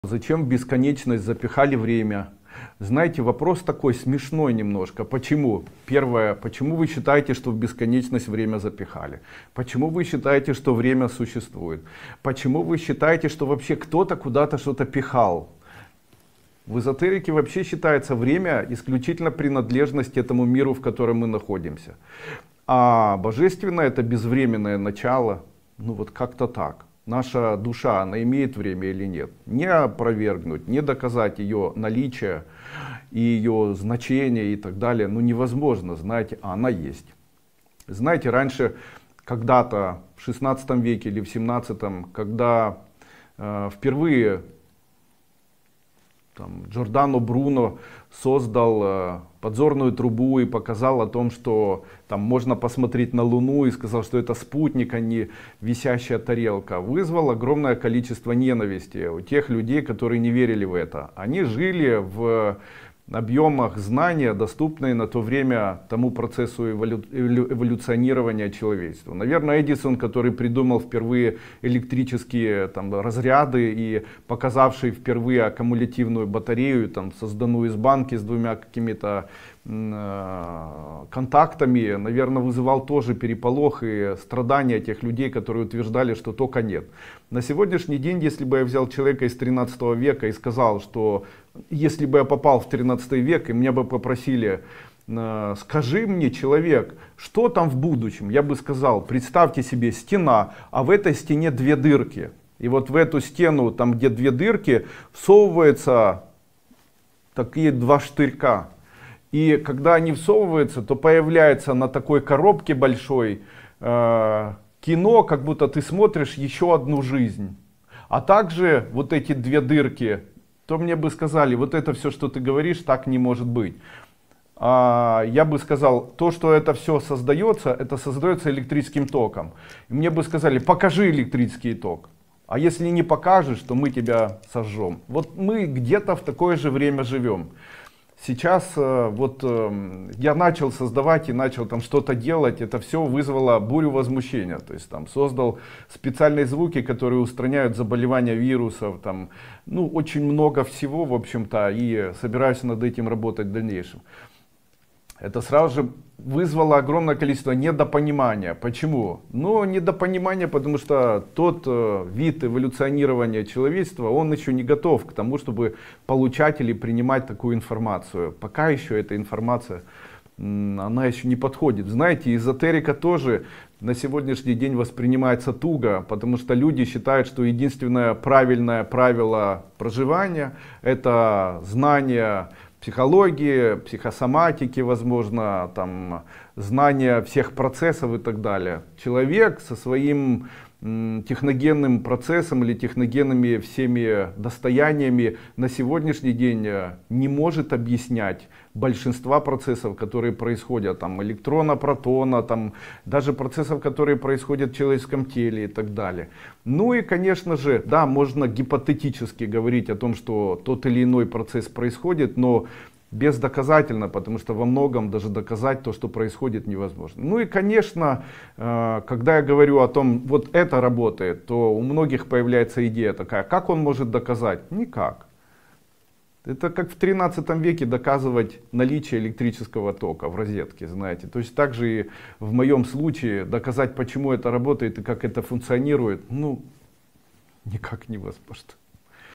Можно ли доказать действие Эзотерики? Существование Души и Времени
Что если время – это всего лишь иллюзия, а за его пределами скрыта бесконечность? В этом видео мы исследуем вопросы, которые тревожат человечество веками: можно ли доказать существование души? Как время связано с нашим физическим миром, и есть ли у души своё собственное измерение? Мы также обсудим эволюционные процессы, которые мы проходим, и выясним, могут ли люди воспринимать эзотерическую информацию. Возможно ли увидеть действие невидимых сил, таких как электричество, и знаем ли мы всё о нашем теле? Погрузитесь в глубокие размышления о божественном, времени и эволюции вместе с нами!